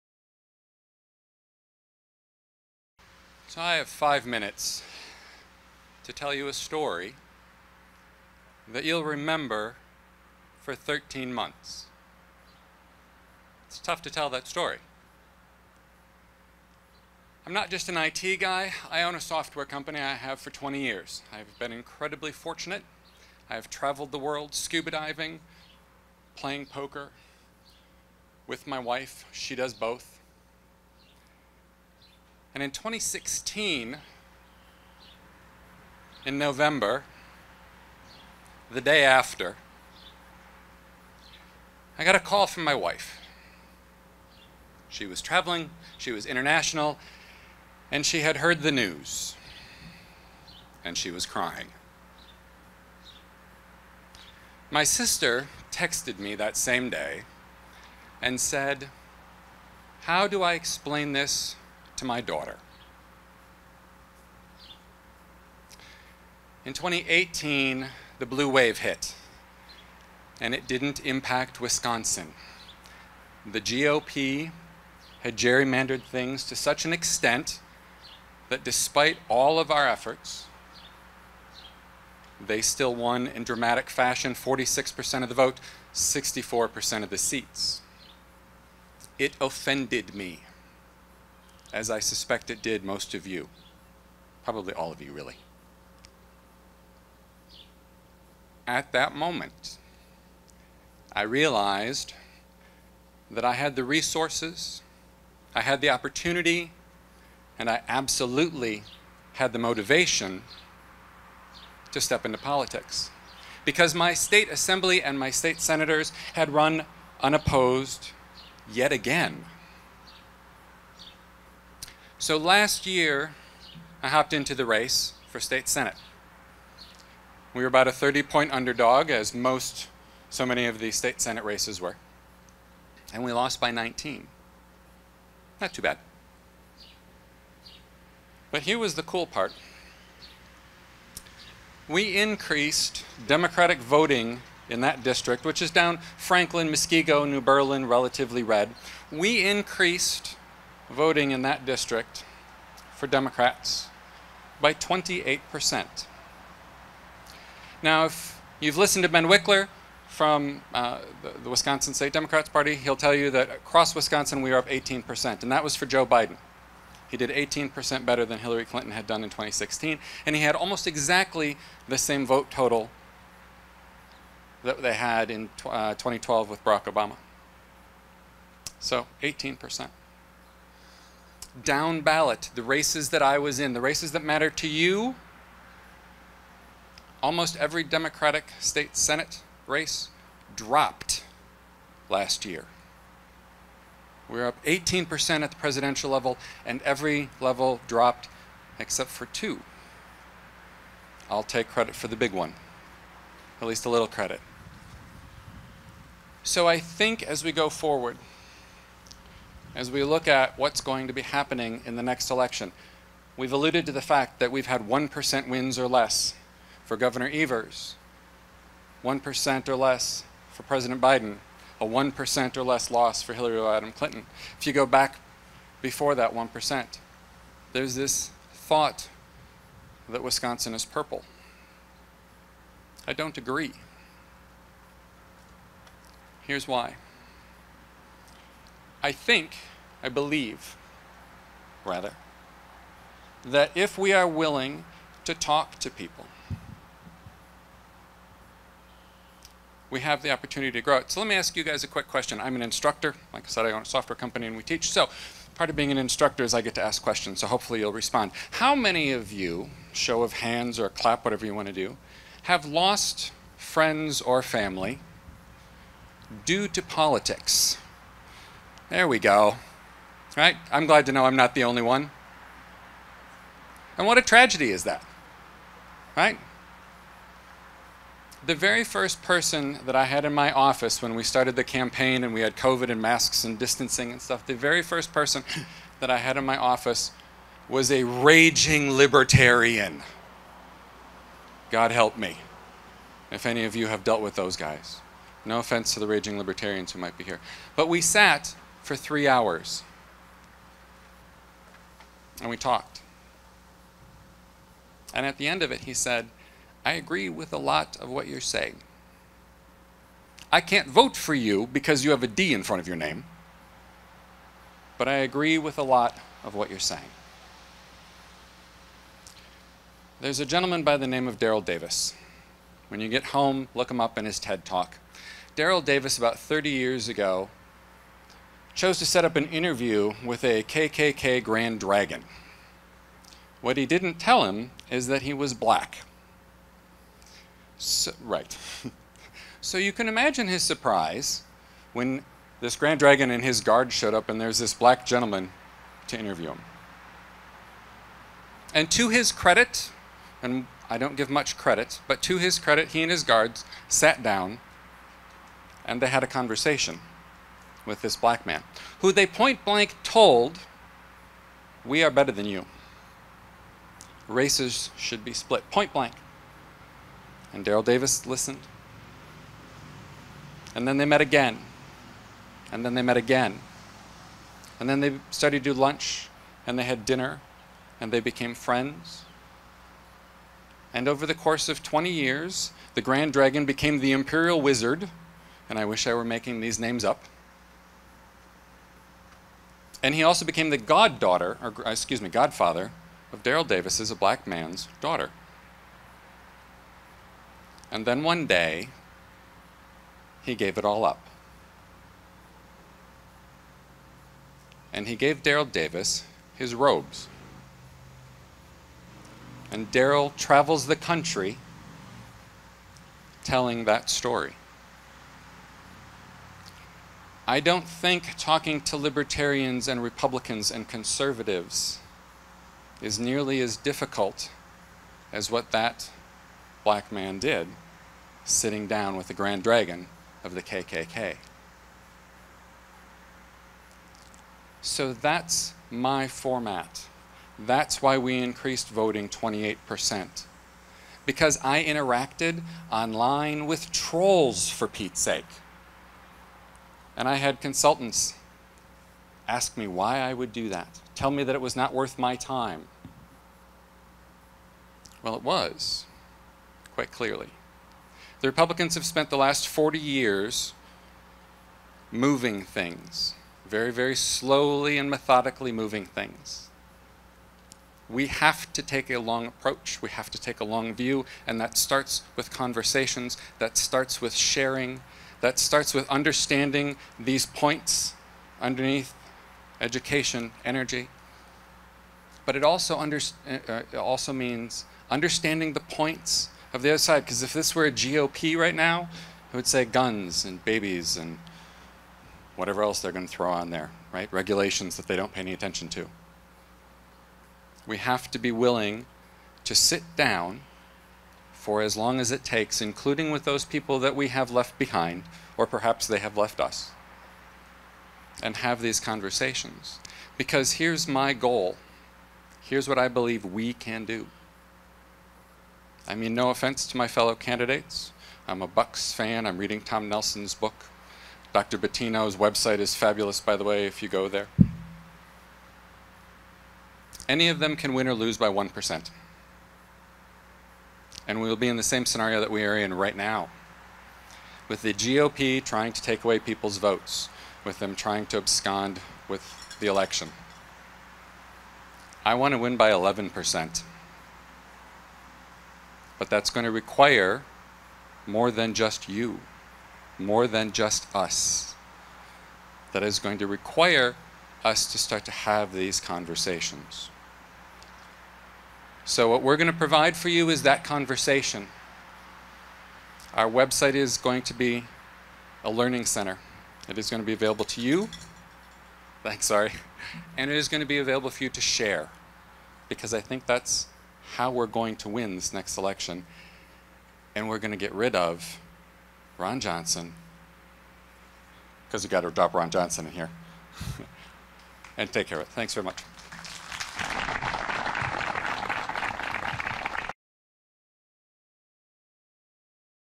Meet the US Senate Candidates with GROW - Adam Murphy
Adam Murphy speaking at the GROW (GrassRoots Organization of Waunakee) Meet the Candidates session.